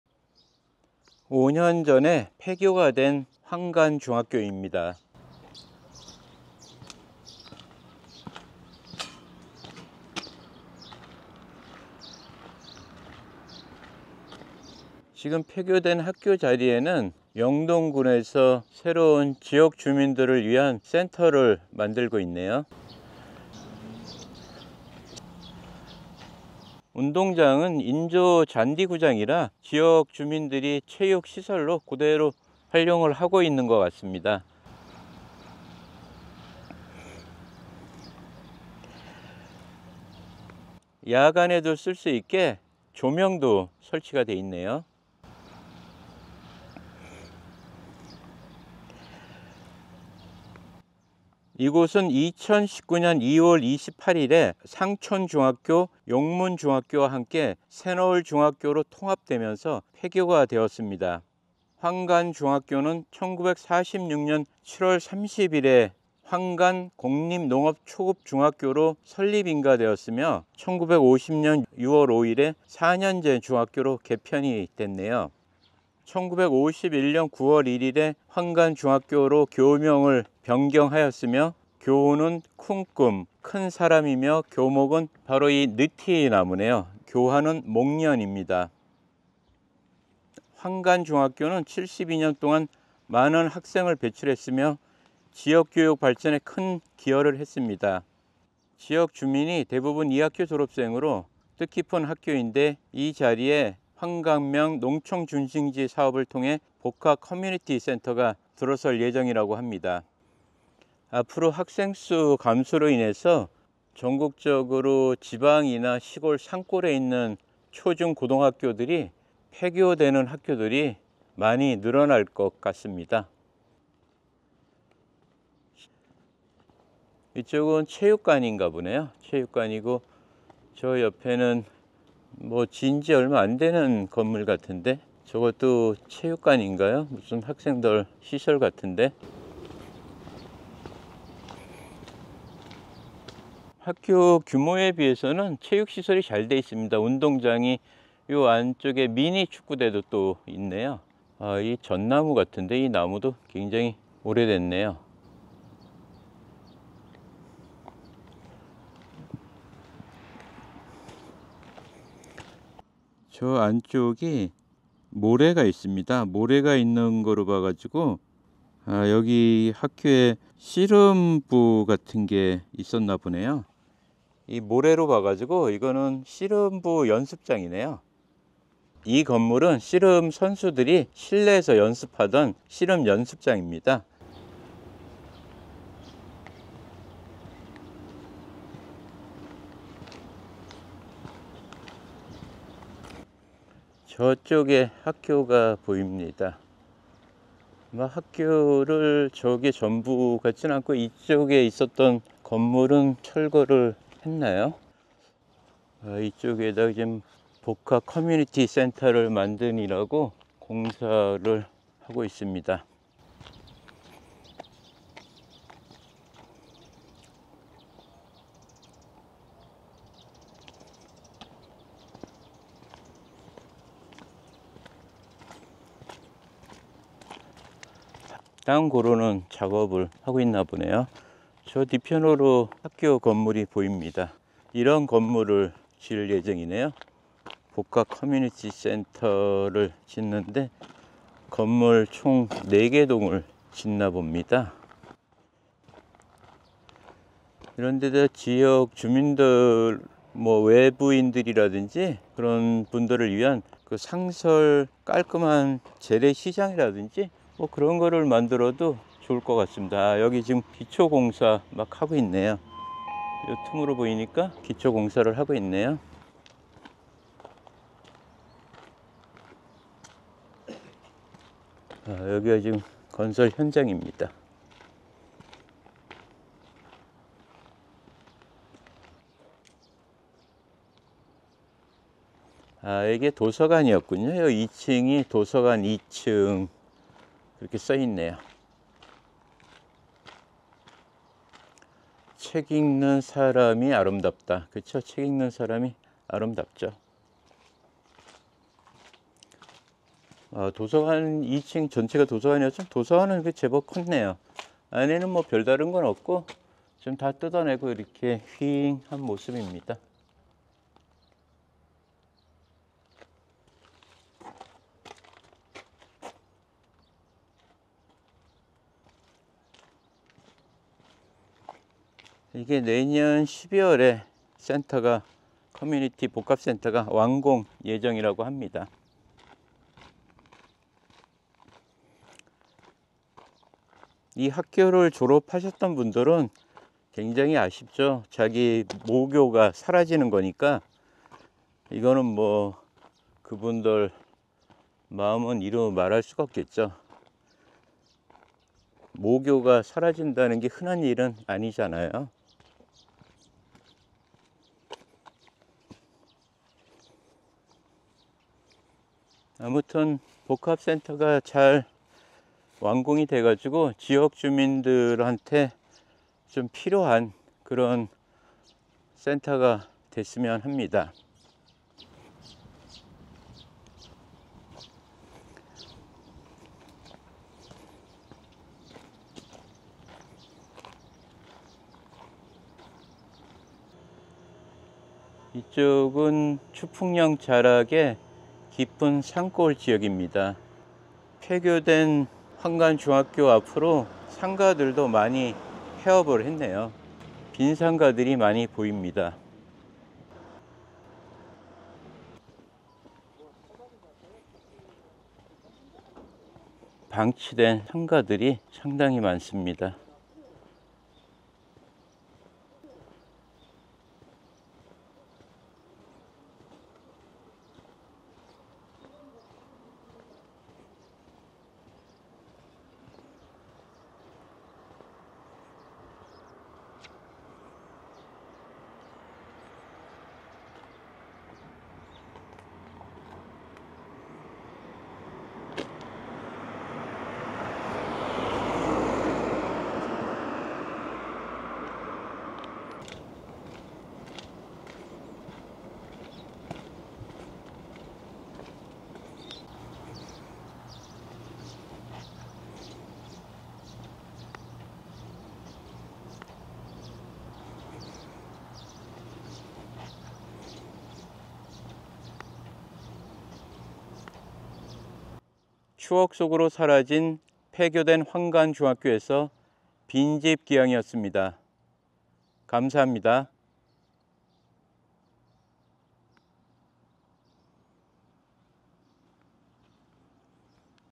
[빈집귀향]대한민국 미래가 큰 일, 학생이 없다
@인구감소로 빠르게 늘어가는 폐교 입니다